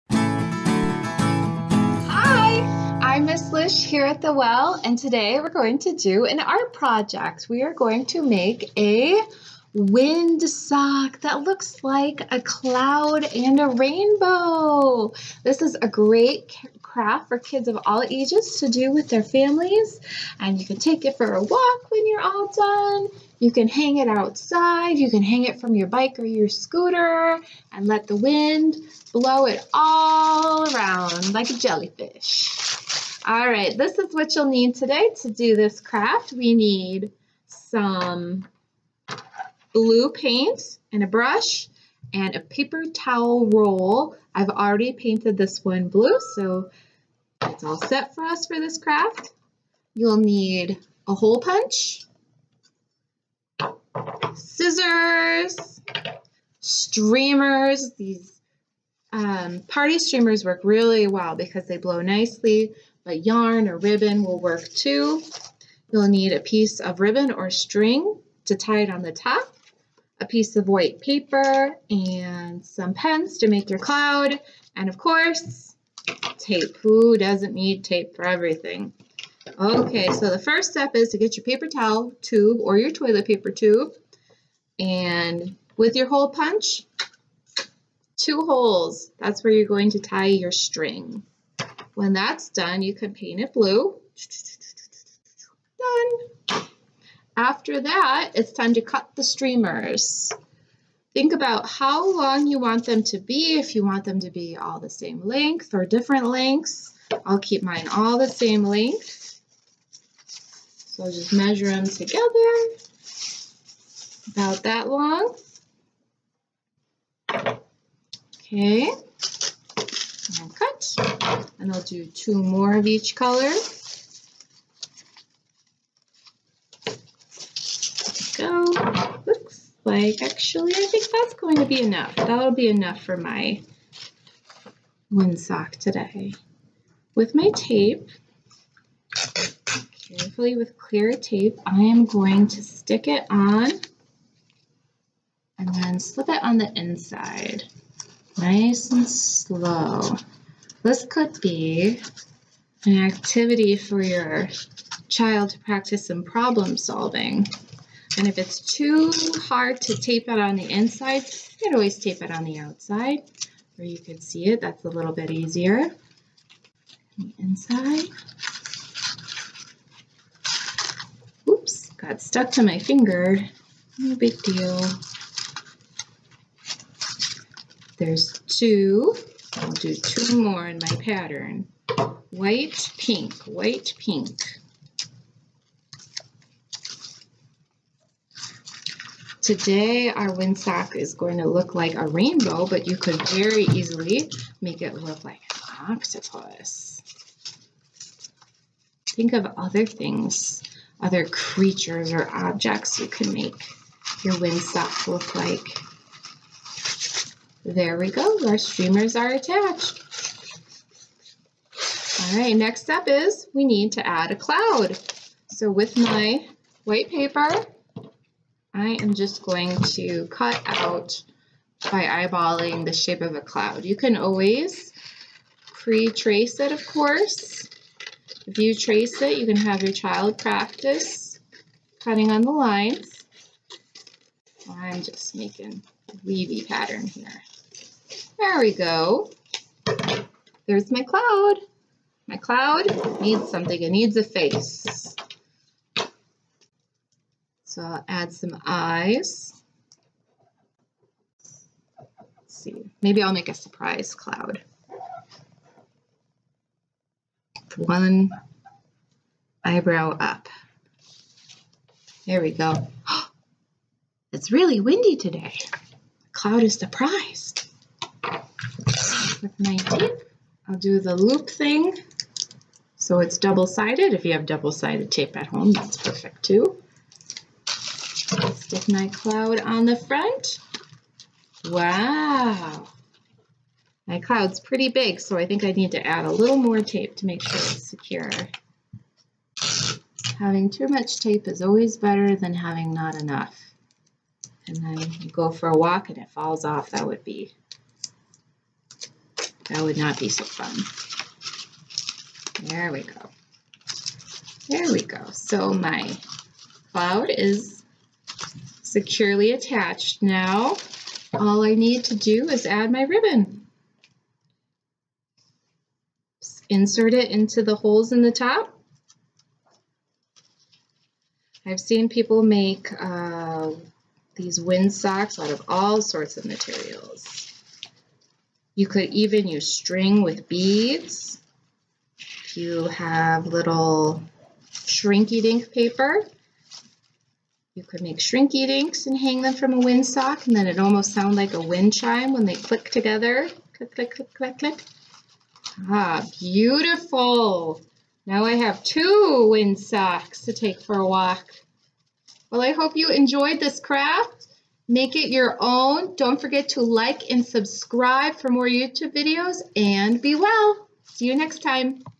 Simple Art for Preschoolers | Rainbow Windsock
Grab a paint brush and put on an art smock, it’s time for Artist Studio with Miss Lish our Teaching & Learning Manager here at The Well.

For this art project you will need;

Blue & White paint
Scissors
Black marker
Tissue paper, strings or streamers in rainbow colors
String for hanging
Hole puncher

Start by painting the toilet paper roll blue, set the toilet paper roll aside so that the paint dries completely while you start working on the cloud. Draw a cloud shape on white paper with a thick line, this will help your Wellies have something large to follow as they cut the cloud out. Once the cloud is all cut out, you can leave it as is, or make a friendly cloud by drawing eyes and mouth.  Next, punch two holes in the paper roll, push yarn / thread through both of the holes and  tie it into a loop. Glue the cloud on the toilet paper roll. Cut long strips out of tissue paper and stick them inside the toilet paper roll. Glue them all the way around and you are done. You’ve made a Rainbow Windsock Toilet Paper Roll Craft.